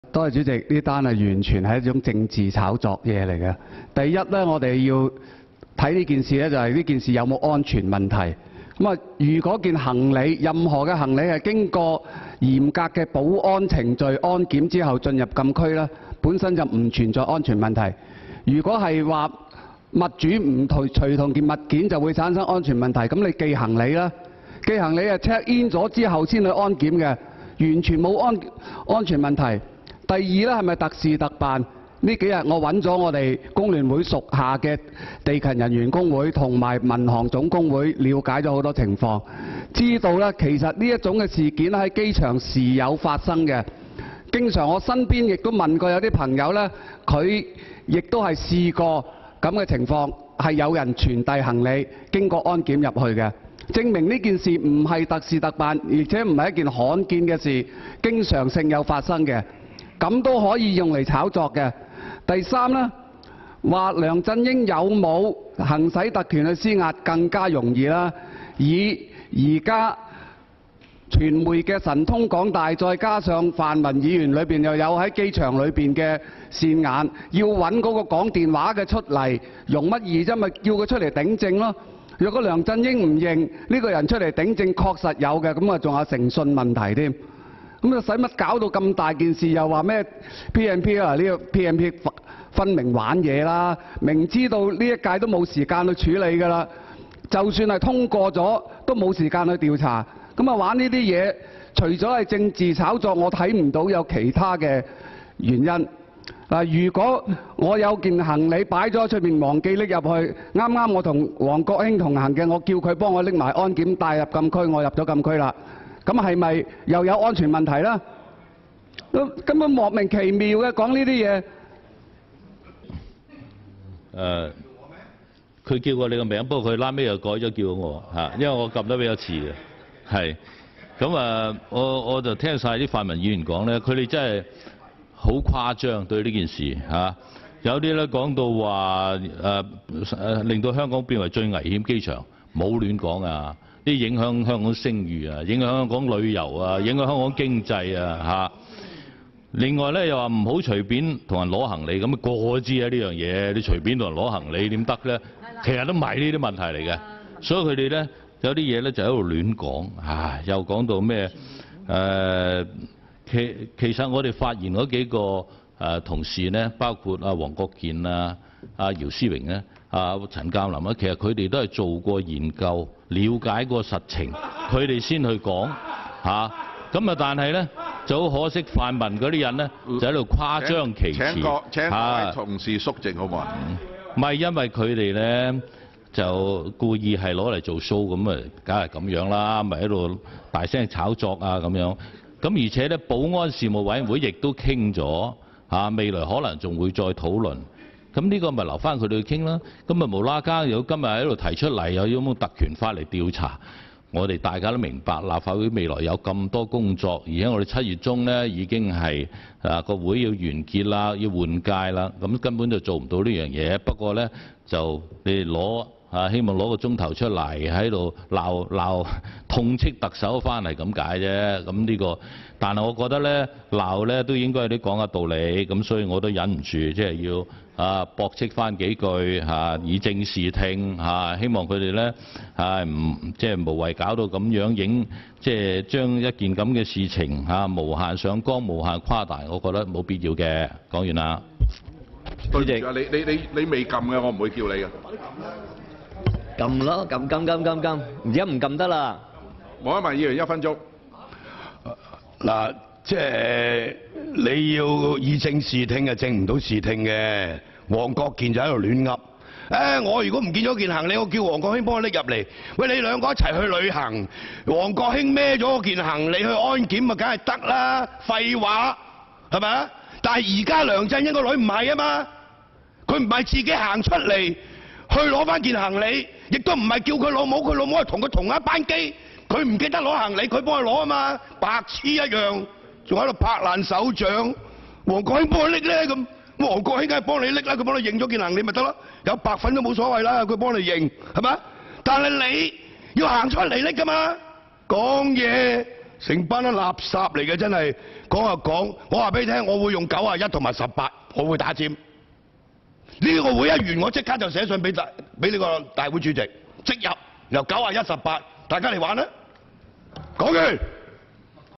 黃毓民怒斥黃國健譚耀宗護CY醜態百出
香港特首梁振英3月27日涉嫌施壓機場保安為女兒拿行李，民主派立法會議員提出用。